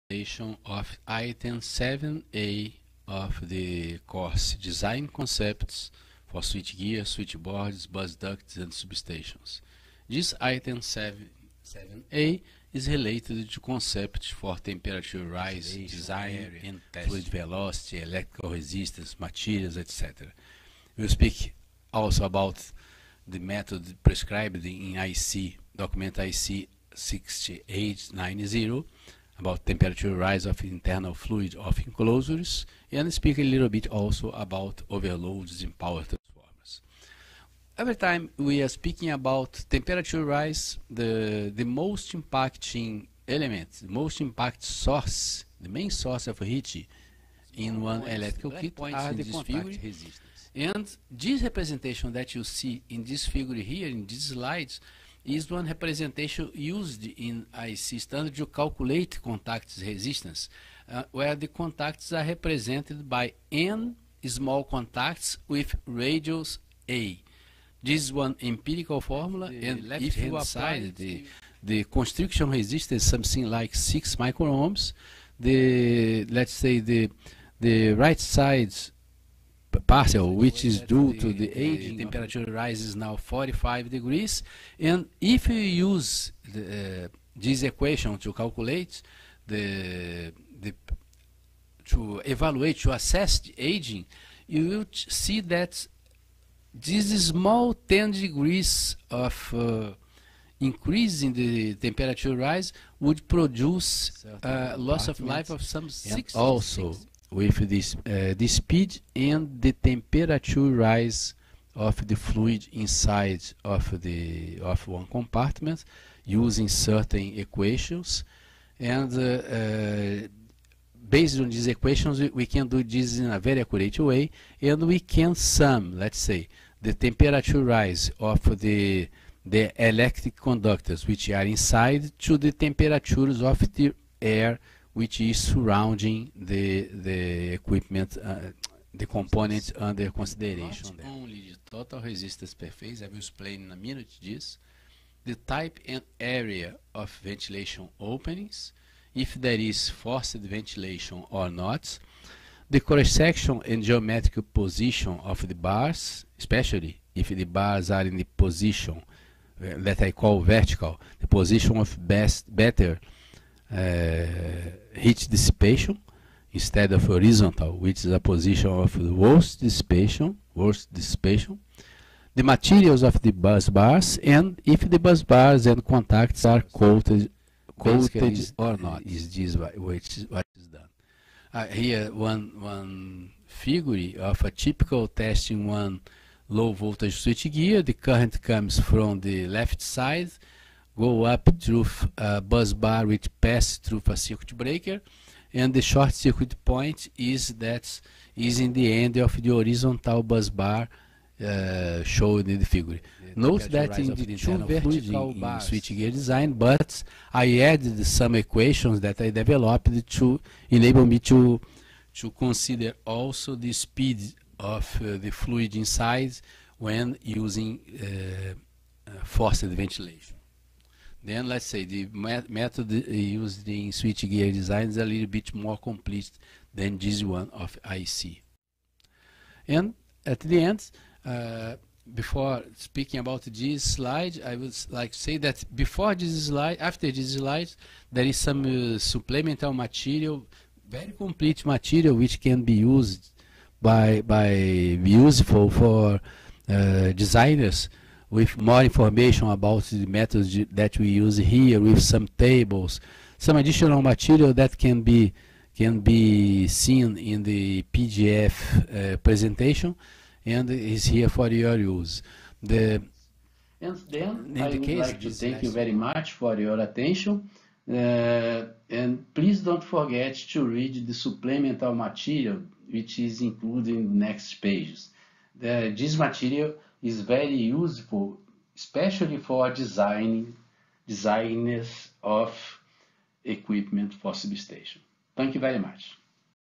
Mini video ... Chapter 7a - TEMPERATURE RISE - DESIGN CONCEPTS & TEST. (IEC61439 + IEC 62271)
This is the "rarest" document of this training. Temperature rise is the most influential aspect of substation equipment design. Here we cover the concepts in the IEC 62271, IEC 61439, and IEC 60943 standards. They include tests and how to calculate temperature rise, contact resistances, loss of life, thermal losses, and others. The basic principles of temperature rise calculation methods are detailed, as well as testing methods and impacts of materials, types, areas, fluid velocities, etc. Chapter 14 shows how to calculate the internal fluid temperature rise by the method of IEC 60890.

The requirements of IEC 62271-1 are also shown as well as the analysis of the temperature rise limit values of these standards. Includes details on aging mechanisms for contacts and connections and loss of life calculation.